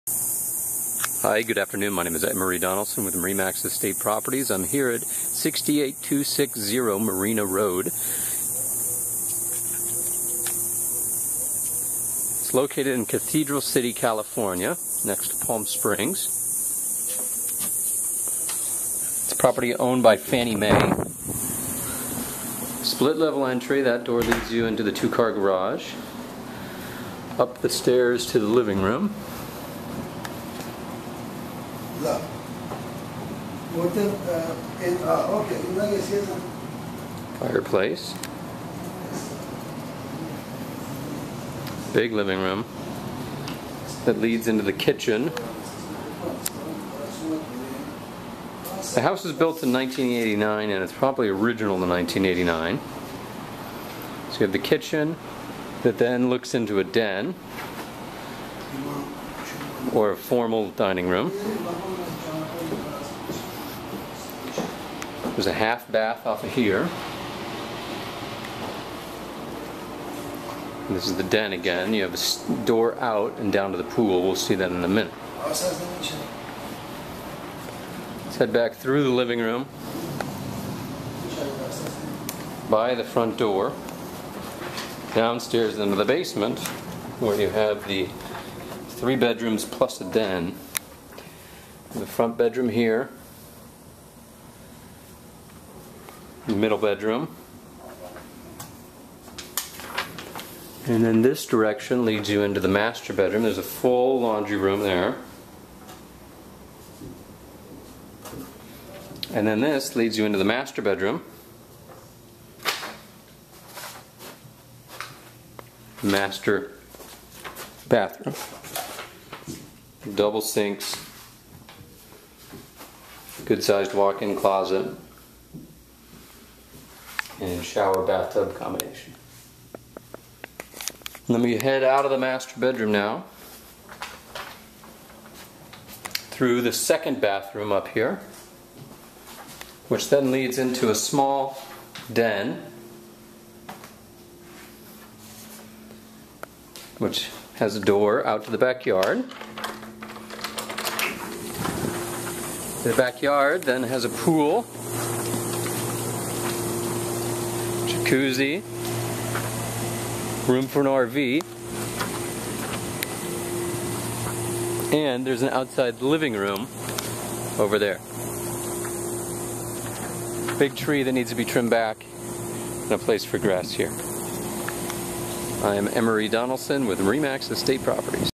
68260 Marina Rd, Cathedral City, CA 92234 -- $255,000
3bedroom/2.5bath split level pool home. Custom paint and new carpet make this home shine. Enter at the 2nd floor with a spacious living area, fireplace, dining area and kitchen and powder bath. Downstairs are the three secondary bedrooms, small play room and two full baths. Backyard has an enclosed patio and pool and spa.

Emory Donelson
Re/Max Estate Properties